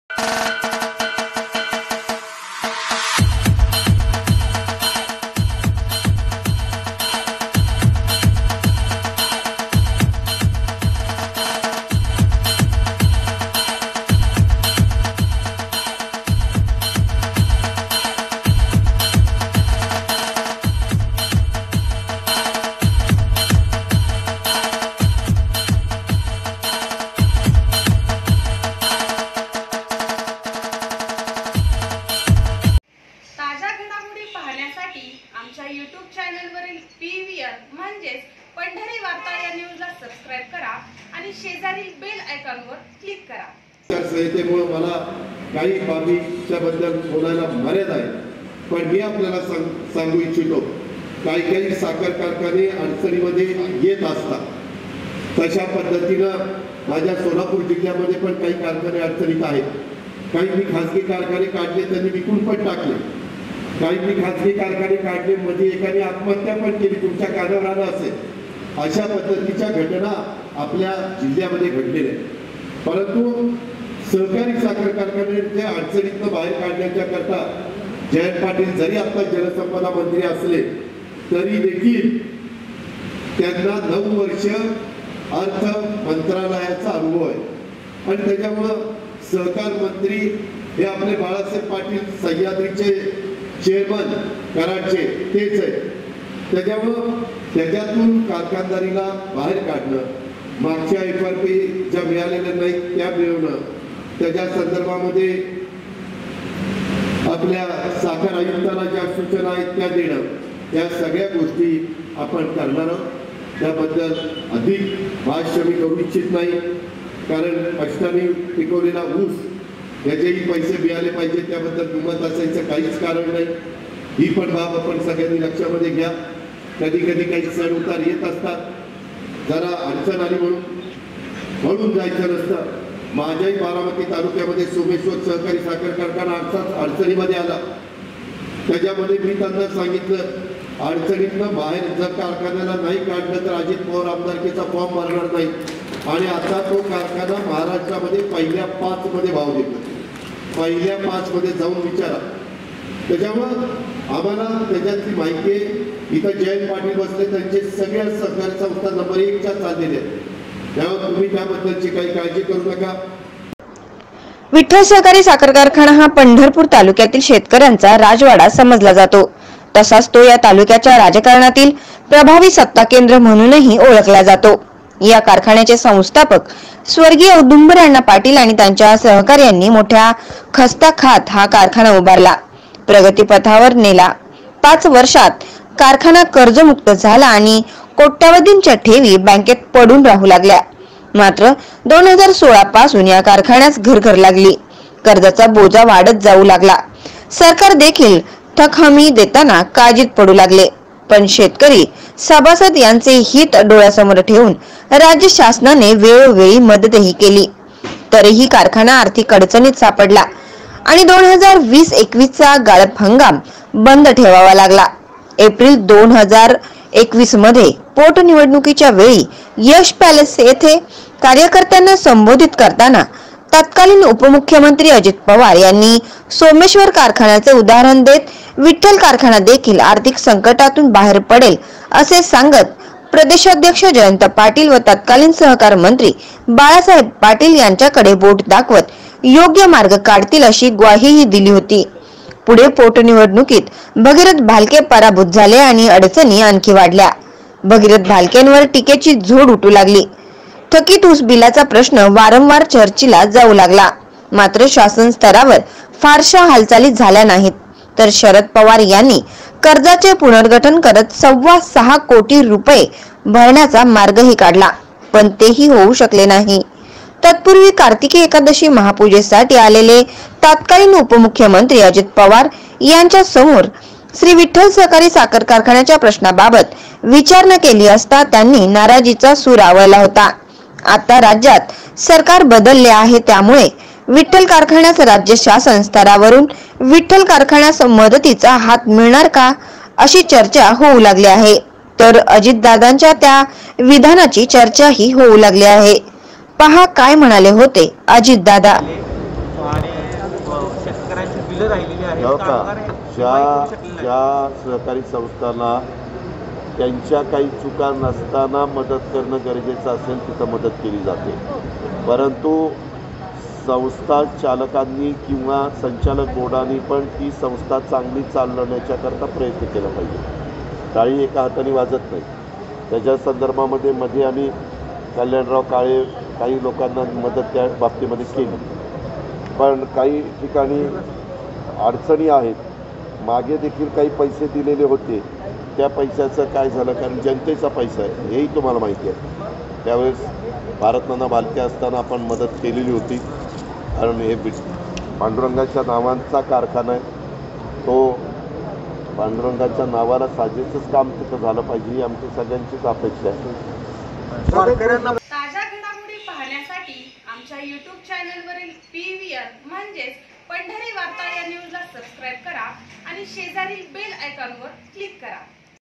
हे पहा.. भारतनाना भालके असताना आपण मदत केली होती !
'त्या' पैशाचे काय झाले ? शेवटी जनतेचा पैसा आहे !
मा.उपमुख्यमंत्री अजित पवार यांच्या 'त्या' विधानाची निवडणुक प्रचारात होऊ लागली चर्चा
राज्यातील सत्तांतरामुळे 'विठ्ठल' समोरील अडचणी वाढणार ?